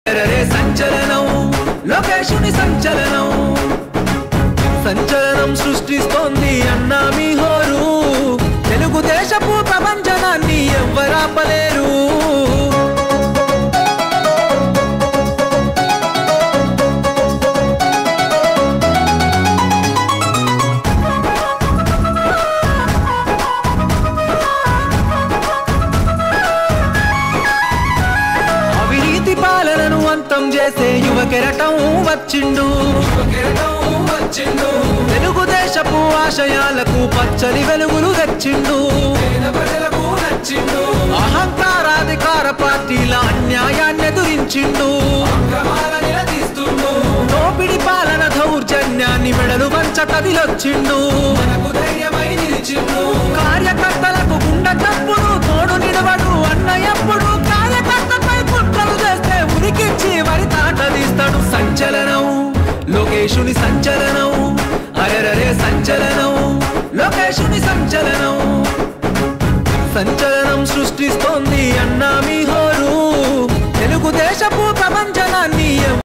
నల్లచెరువు: టిడిపి యువ నాయకుల ఆధ్వర్యంలో నారా లోకేష్ జన్మదిన వేడుకలు ఘనంగా నిర్వహించారు
ncu local news